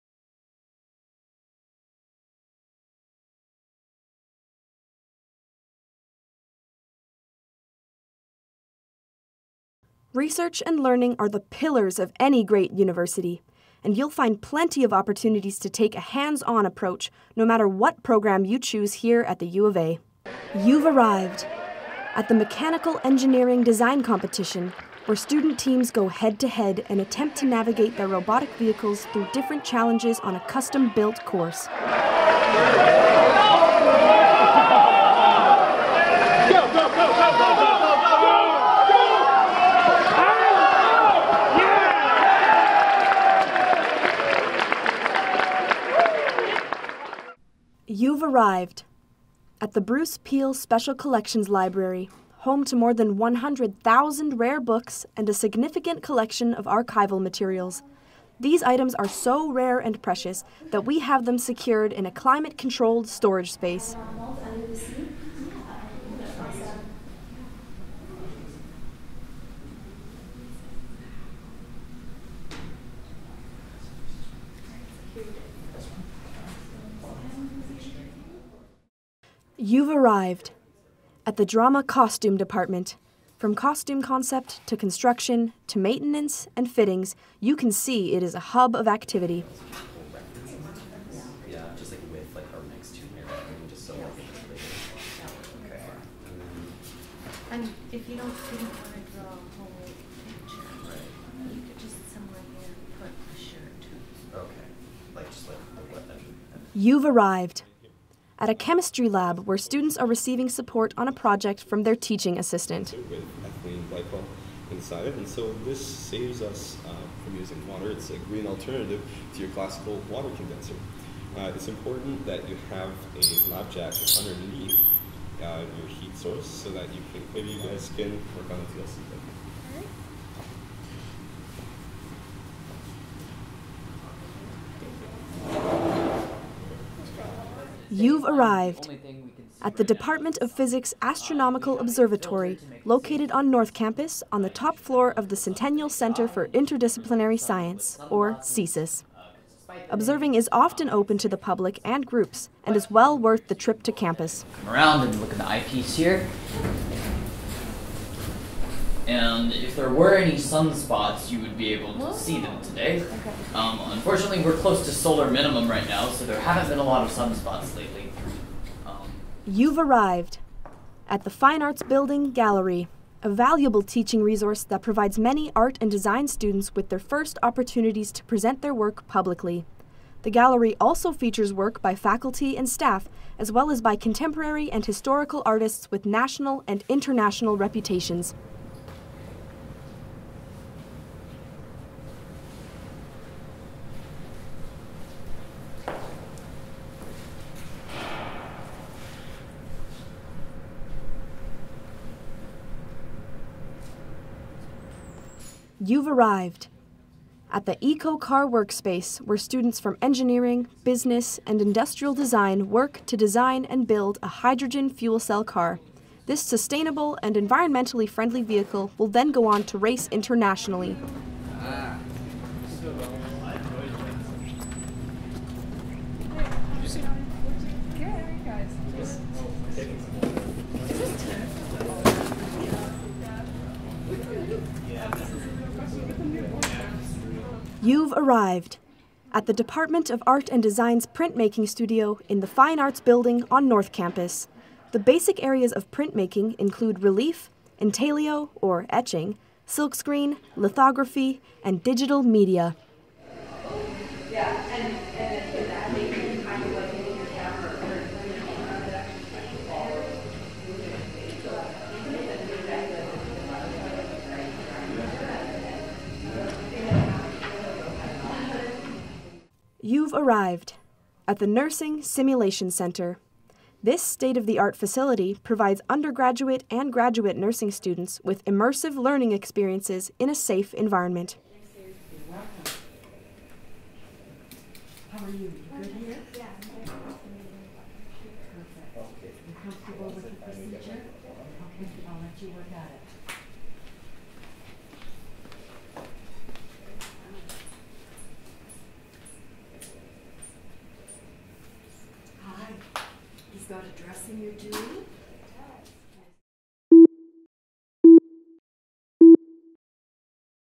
UAlberta 360 Virtual Tour - Research & Learning
Experience some of the spaces on campus where hands-on learning takes place! In this section you will see:

Mechanical Engineering Design Competition - 0:10
Bruce Peel Special Collections - 0:47
Drama Costume Department - 1:18
Gunning/Lemieux Chemistry Centre - 1:45
Astronomical Observatory - 2:16
FAB Gallery - 2:48
EcoCar Workspace - 3:25
Printmaking Studio - 3:57
Nursing Simulation Centre - 4:28